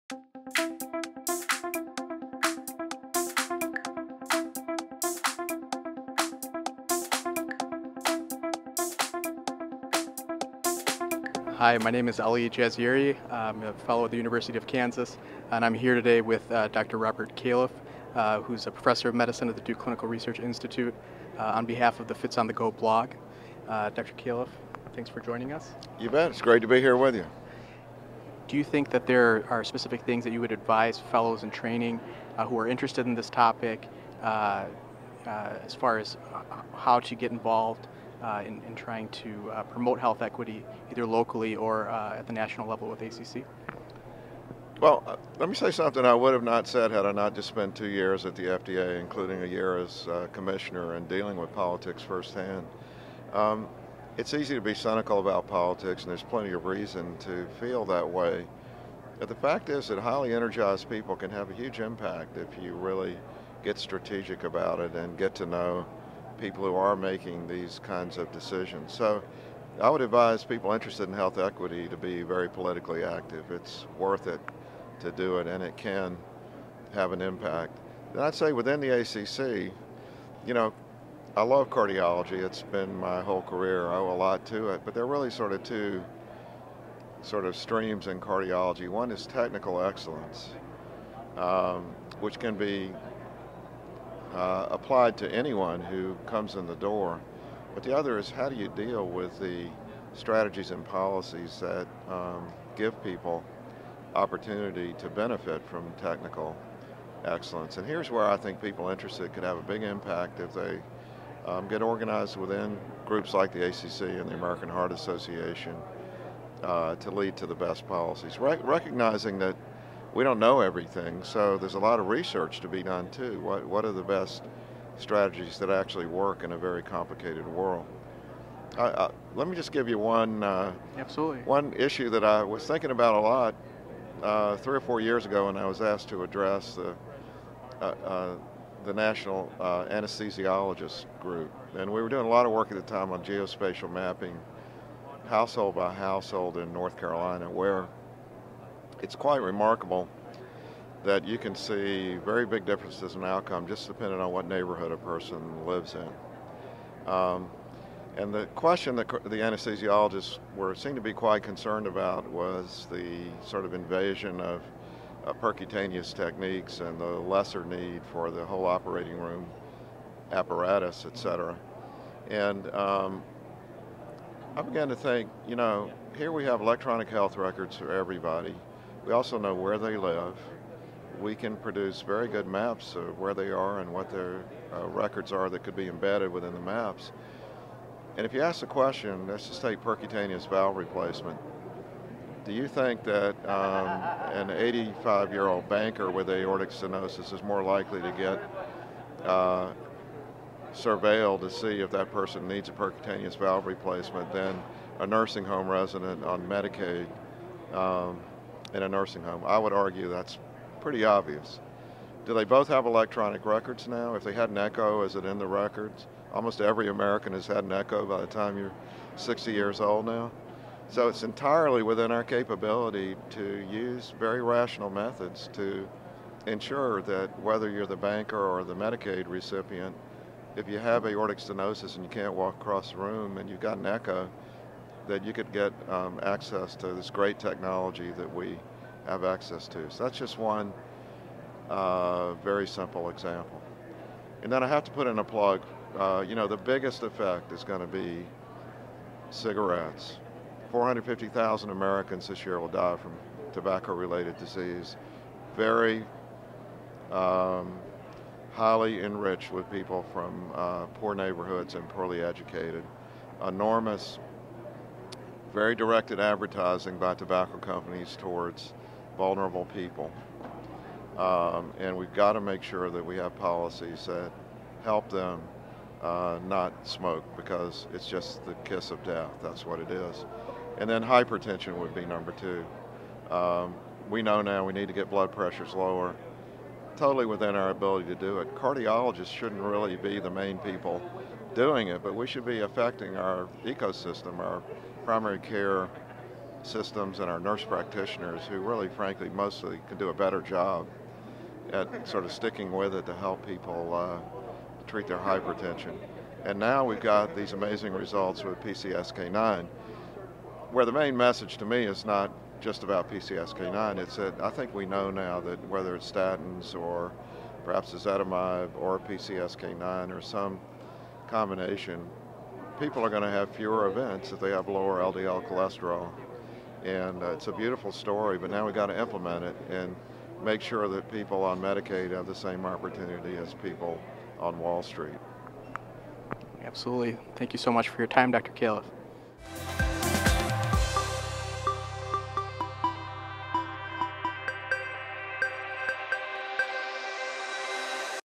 Dr. Robert Califf advises FITs on Health Care Disparities
Former FDA Commissioner